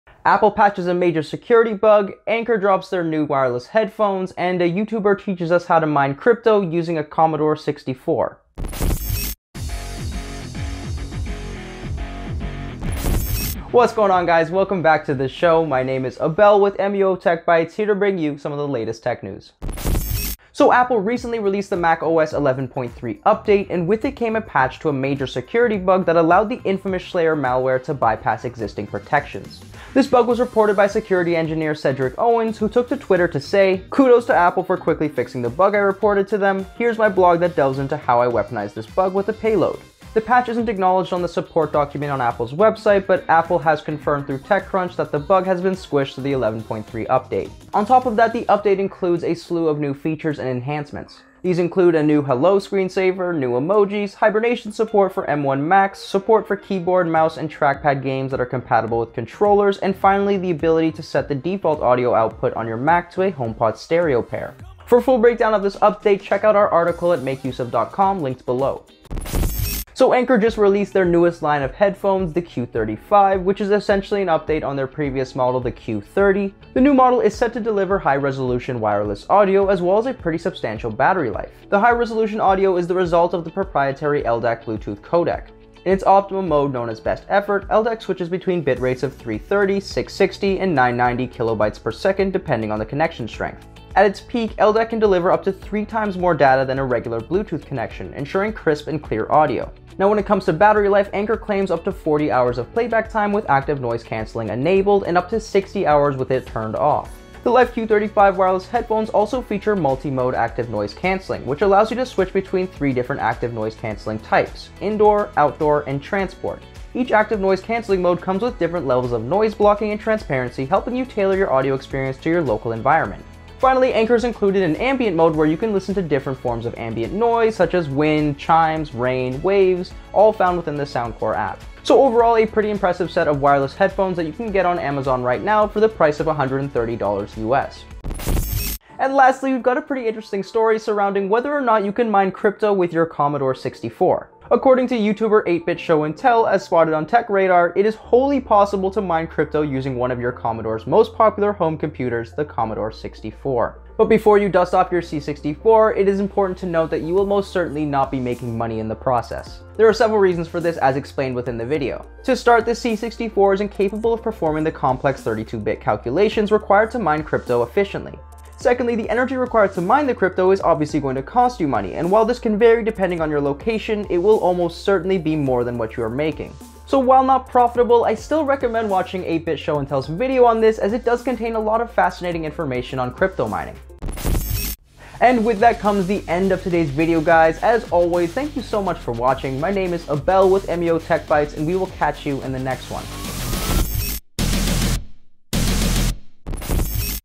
Crypto Mining on a Commodore 64... It's a Thing (Anker Headphones, macOS Security)
In today’s episode of MUO Tech Bytes, we take a bite out of the biggest tech stories from the last 24 hours.

1. Install macOS Big Sur 11.3 ASAP to Protect Your Mac Against the Shlayer Malware
2. Anker Launches New Life Q35 High-Res Wireless Headphones
3. It Turns Out You Can Even Mine Crypto With a Commodore 64

Make sure to Like the video and check back every weekday at 5 PM Eastern for a new episode.